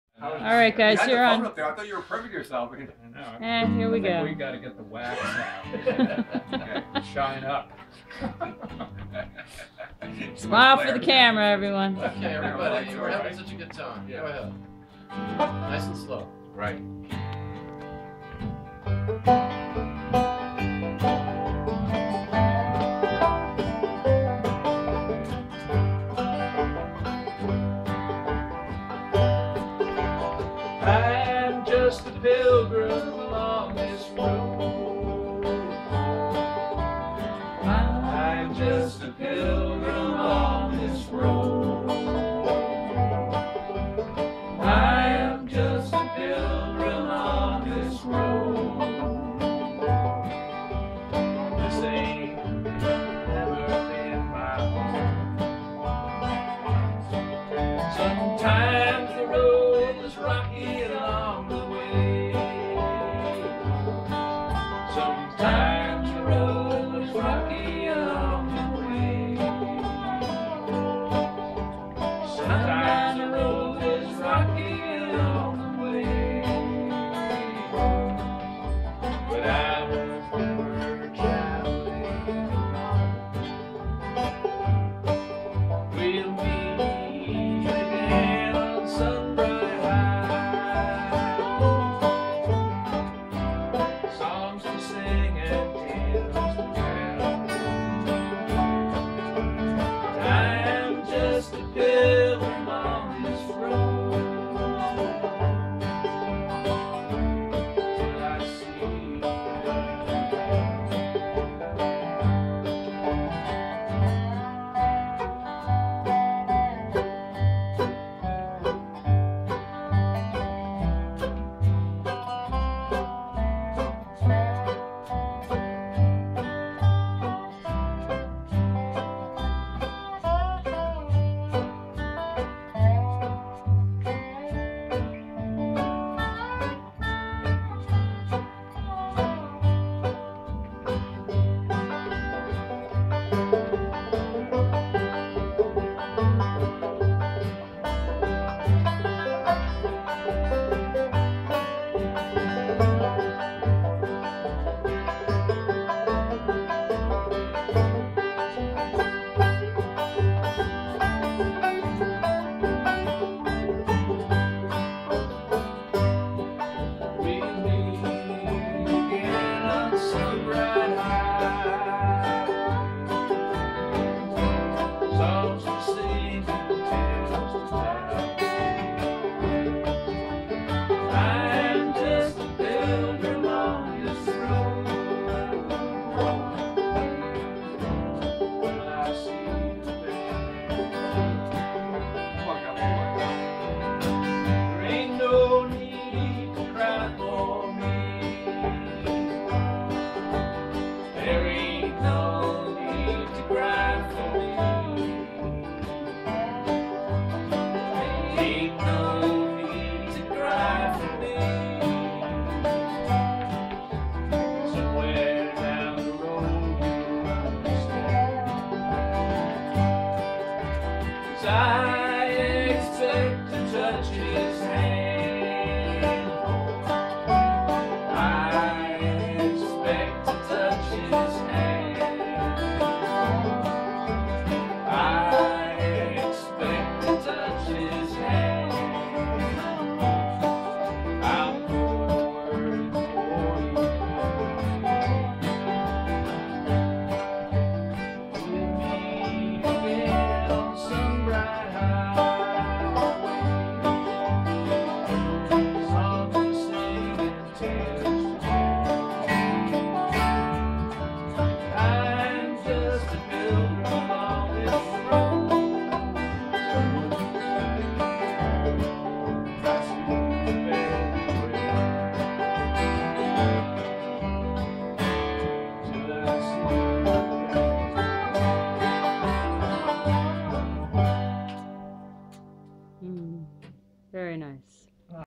Yeah! Special Underkünt Gathering
Michael on dobro, Brian an banjo, Mike on bass and Kevin on guitar.